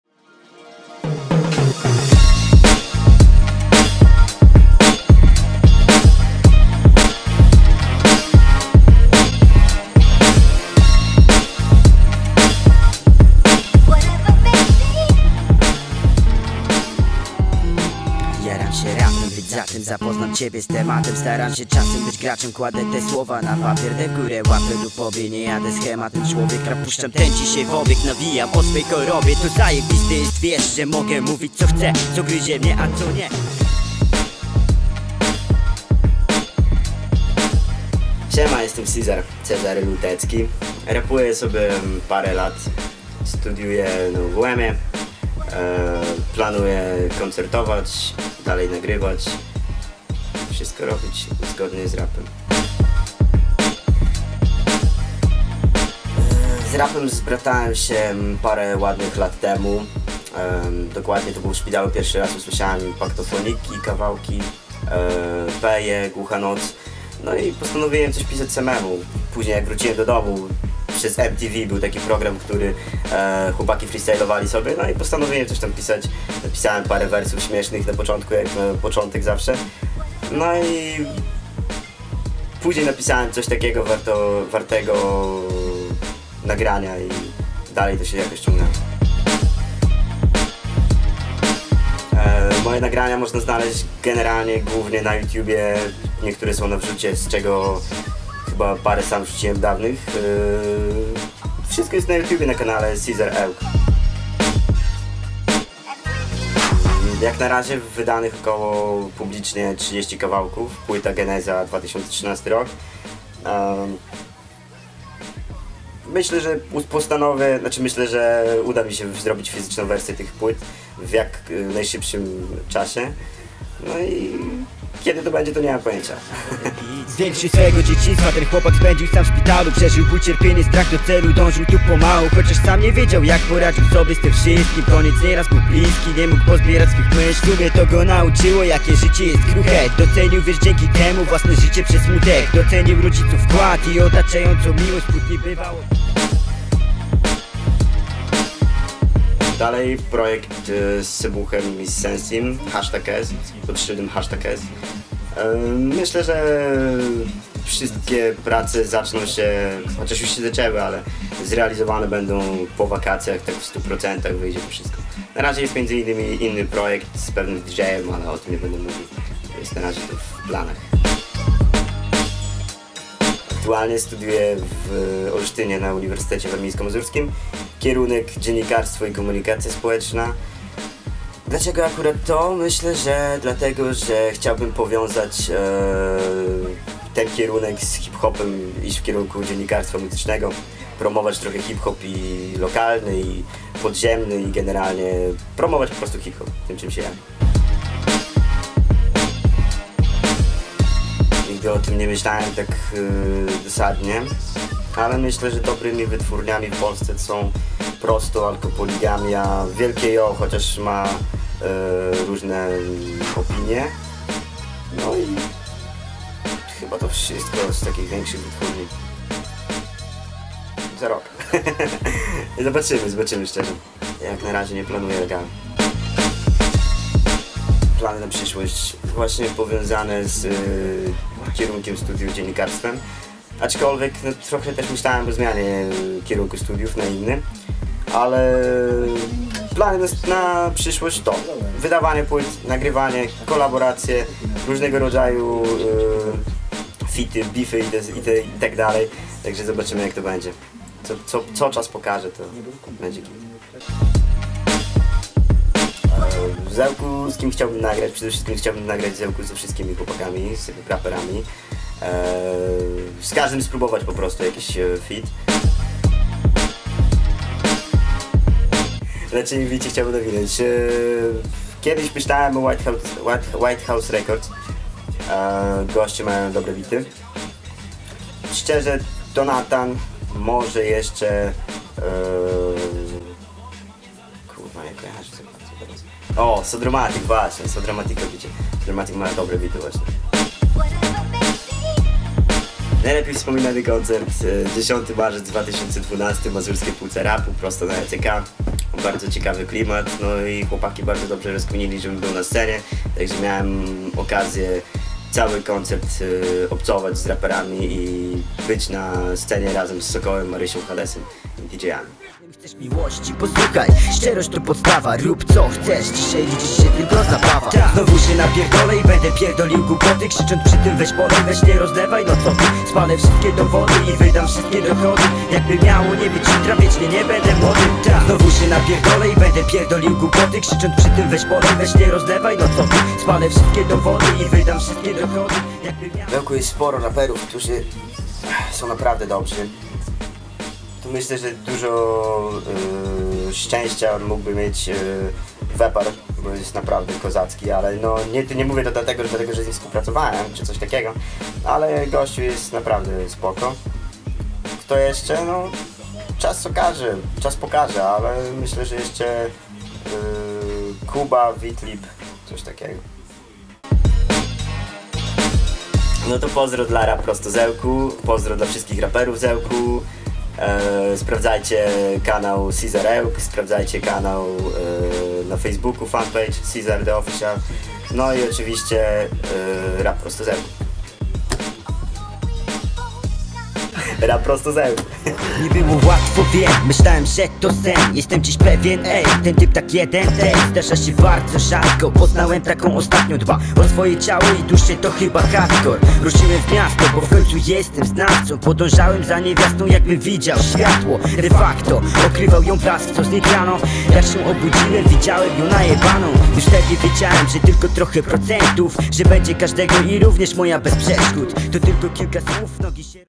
Slam Kanapa - S1O1 - Sizar
Slam Kanapa - Seria wywiadów z reprezentantami Ełckiej sceny pod szyldem RPZE.
Gościem Slam Kanapy jest Cezary "Sizar" Lutecki.

Podziękowania: Slam City, Biger, Sizar, WZK.